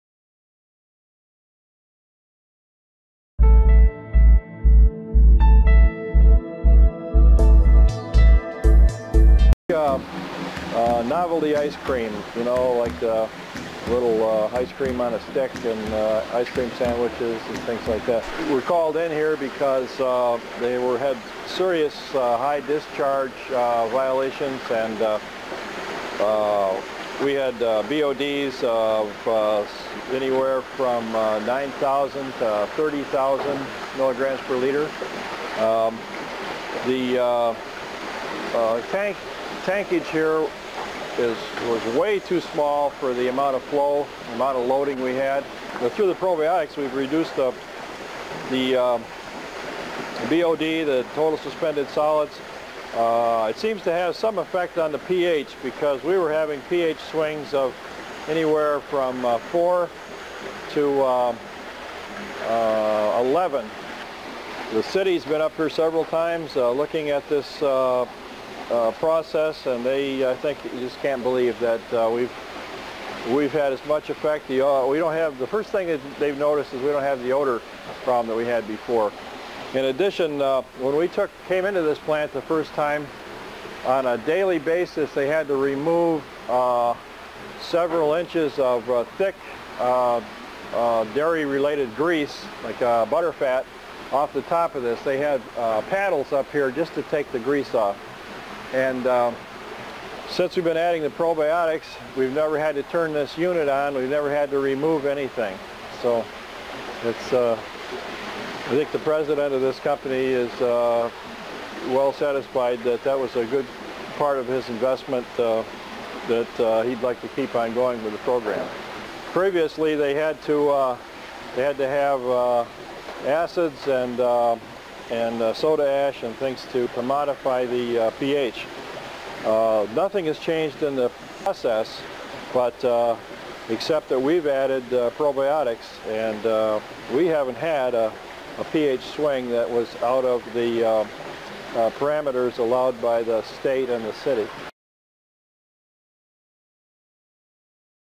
Cleaning Wastewater in Food Processing Plant
Probiotic Solutions helps clean wastewater in Food Processing Plant (ice cream).